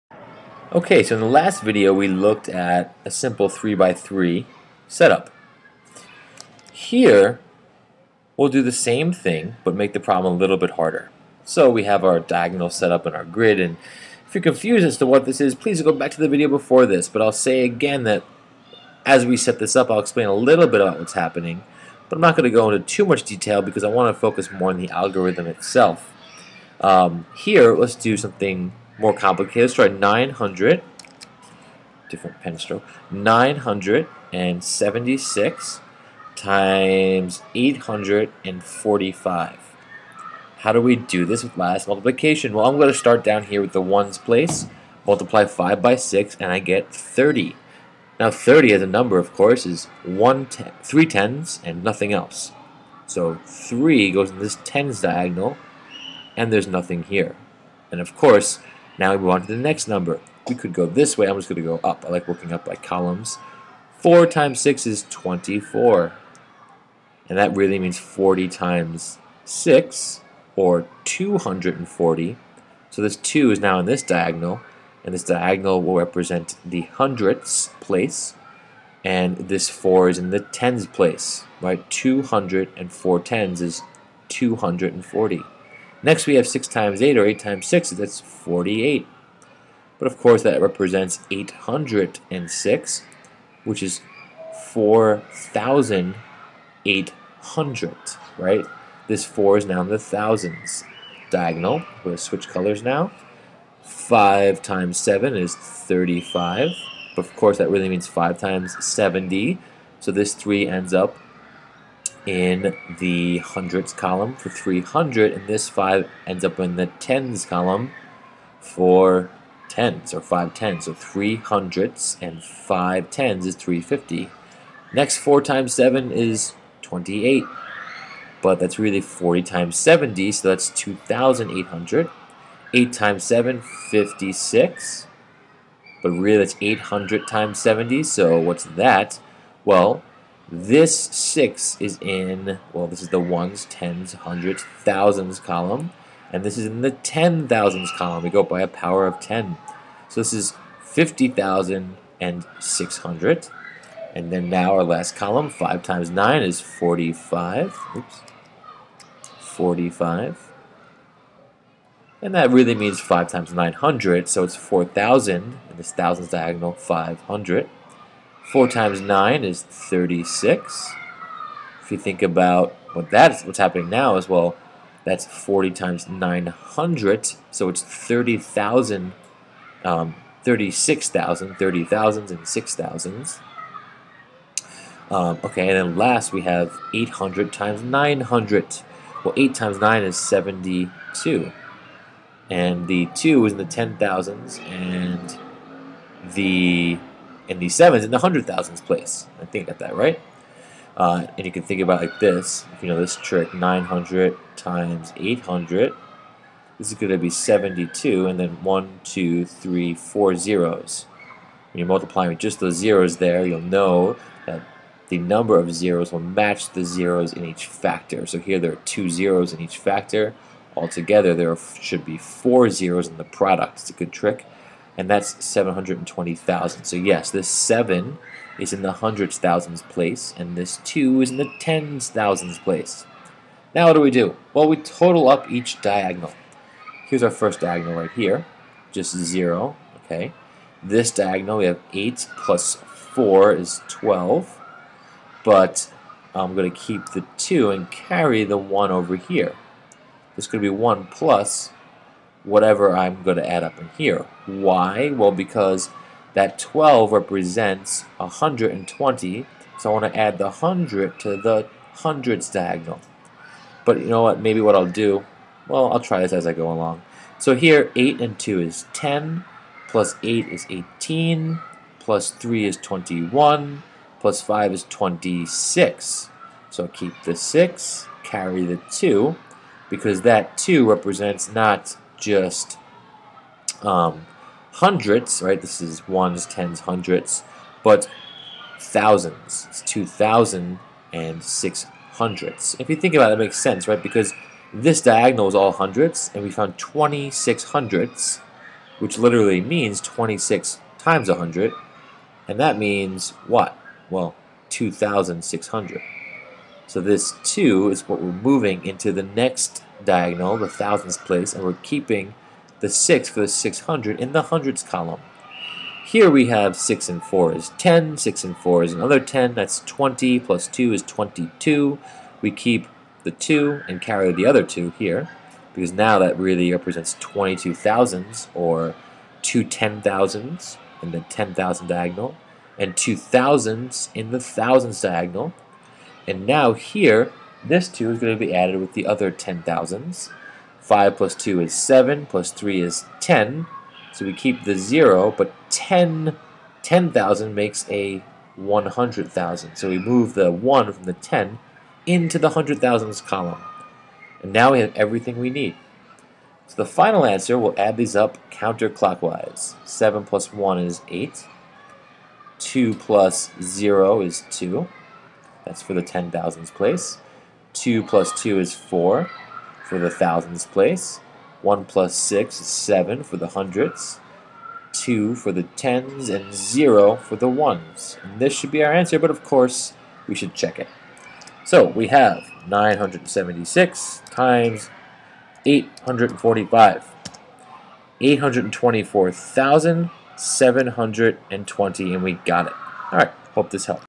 Lattice Multiplication 3 x 3 with carrying
A more challenging example of a three digit lattice multiplication problem